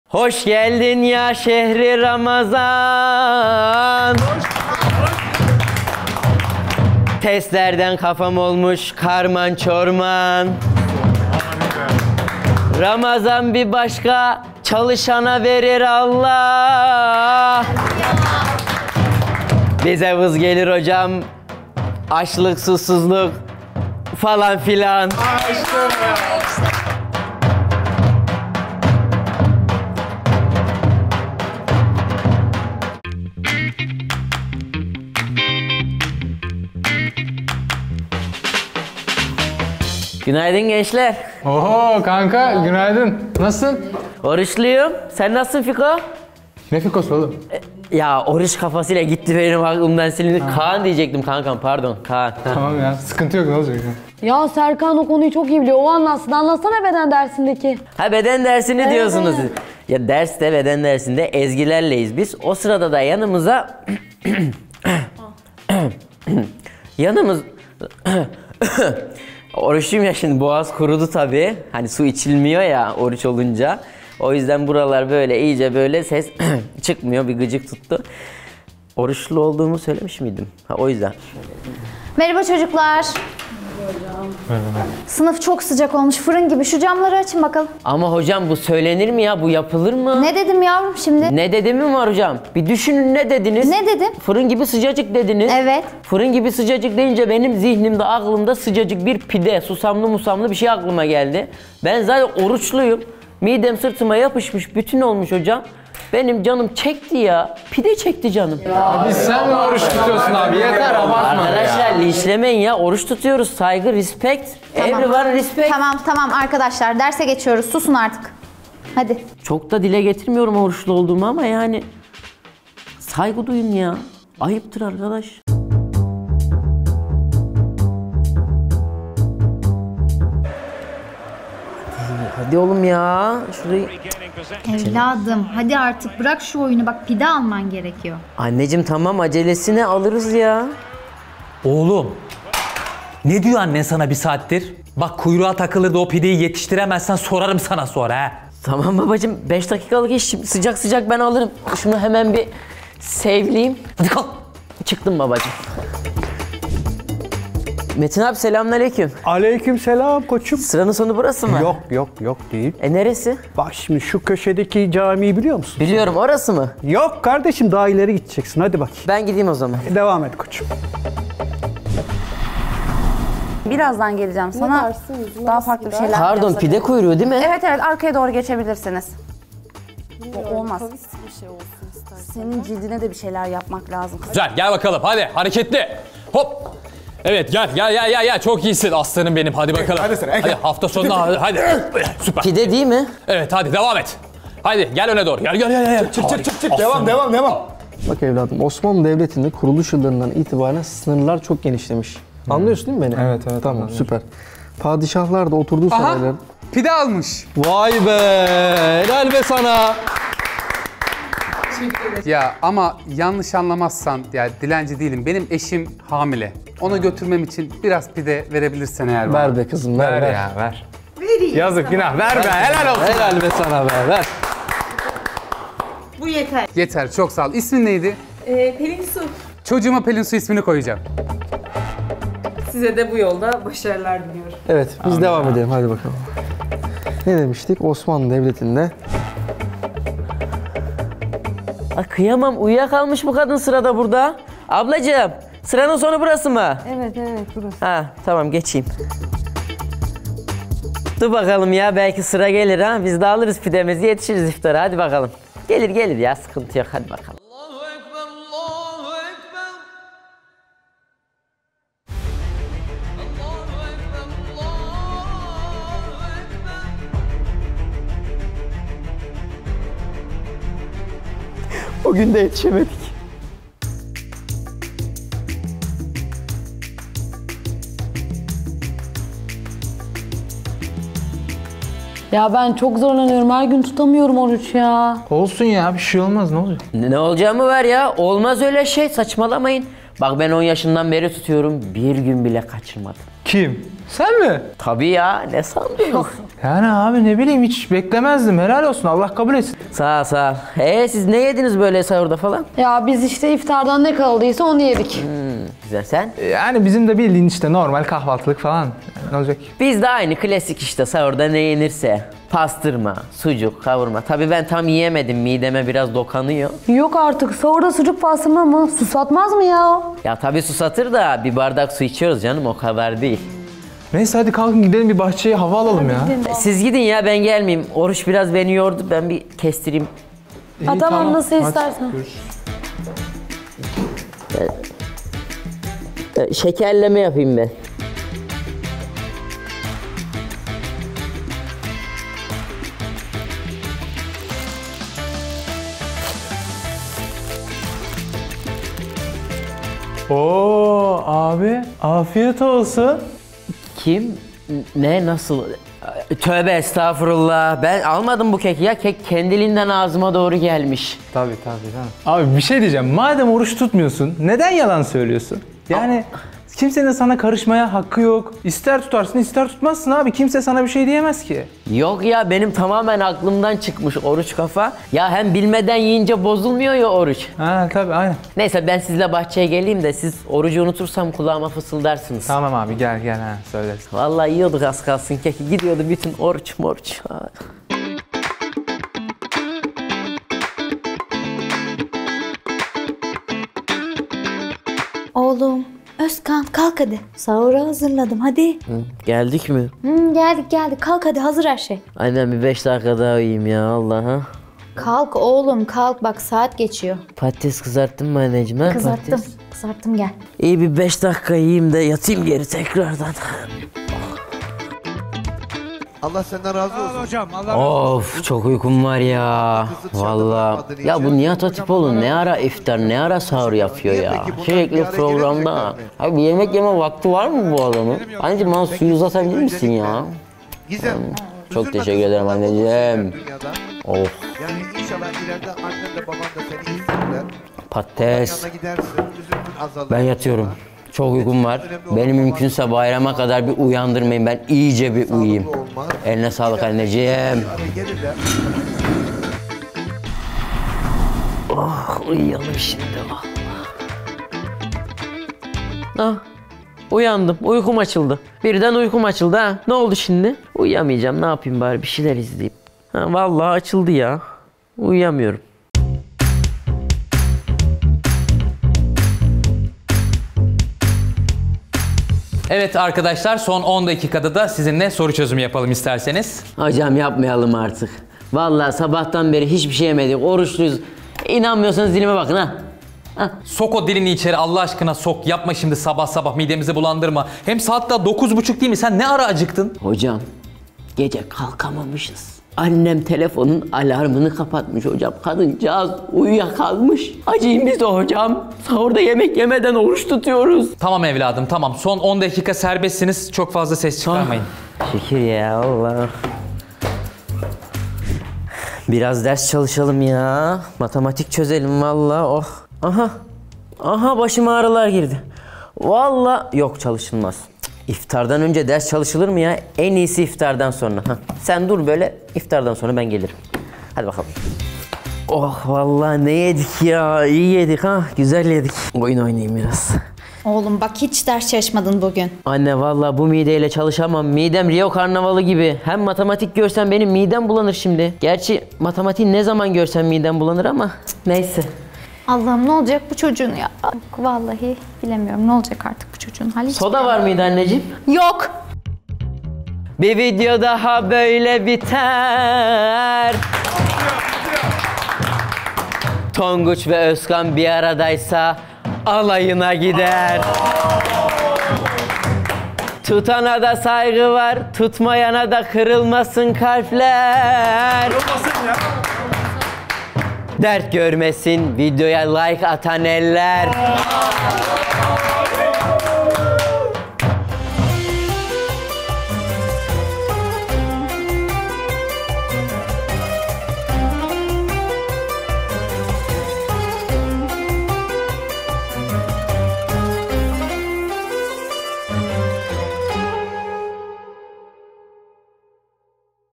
Ramazan Ayında Gördüğümüz 5 Öğrenci Tipi 😀 w/@ozkansagin
Ramazan demek hem uykuya hem açlığa direnmek demek. 😅 Peki sen hangisisin ? 😉Yorumlara yazmayı unutma 👇